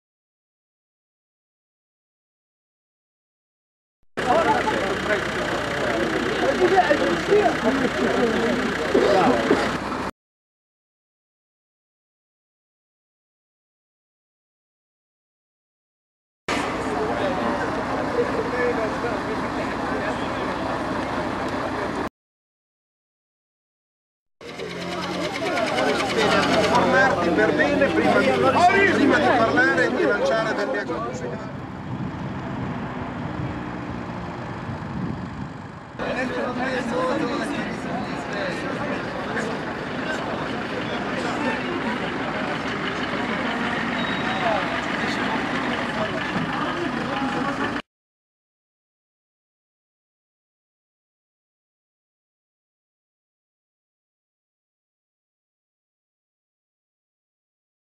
Sciopero della logistica. Continuano i blocchi all'interporto di Bologna
Nella giornata di sciopero nazionale degli operai della logistica continuano i blocchi selvaggi agli ingressi dell'interporto di Bologna.